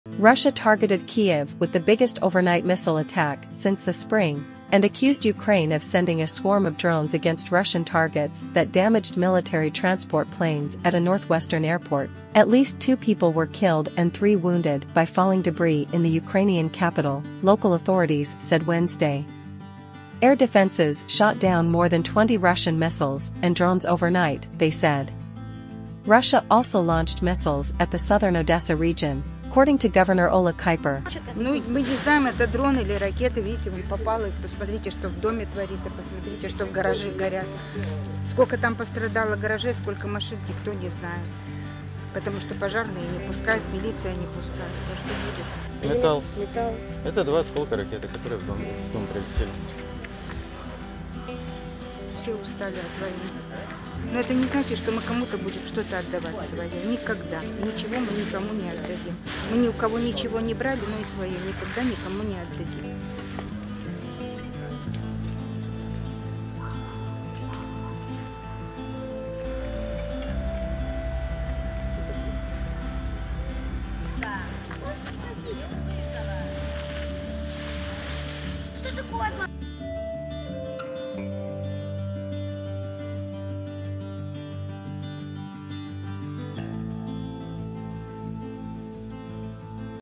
Ukrainian Drones Strike Deep Inside Russia, Hitting Military Airfield, Other Targets
The attacks came as Ukraine reported its troops pierced the first line of Russian fortifications in the nation’s southeast and are fighting to widen the breach. The Ukrainian counteroffensive, started in June, has been bogged down by Russian forces dug in along a vast front line stretching from the Donbas region in Ukraine’s east through the south to the mouth of the Dnipro river. The slow-moving push has worried Ukraine’s allies, who say a long fight risks giving Russian President Vladimir Putin the upper hand in a war of attrition.